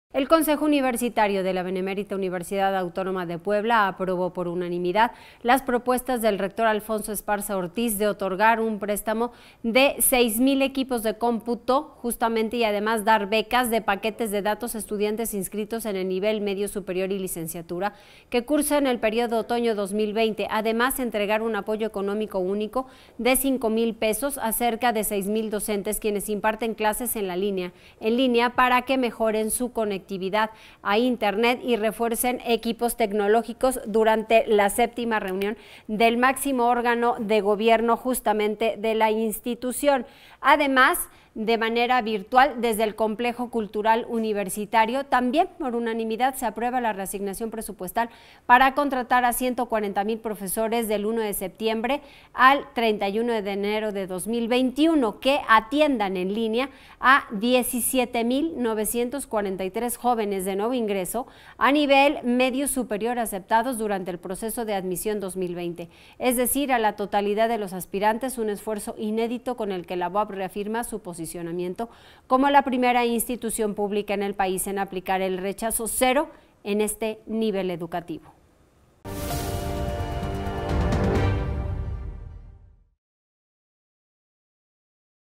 Apoyos BUAP | Las Noticias Puebla
El consejo universitario de la BUAP aprobó por unanimidad las propuestas del rector Alfonso Esparza Ortíz de otorgar un préstamo de 6 mil equipos de cómputo a estudiantes inscritos en el nivel medio superior.